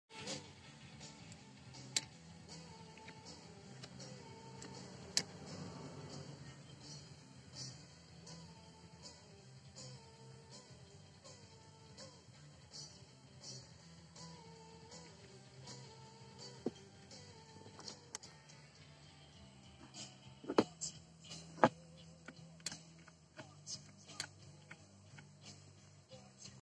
Toyota Corolla 2004 headlight switch / stalk problem (no low beam)
Toyota Corolla 2004 headlight switch / stalk problem.

If you have no low beam, look at your switch / stalk. Probably that copper connector is loose or dirty. Bend it gently, clean it and you're back in the game again.

Super easy to fix, save money.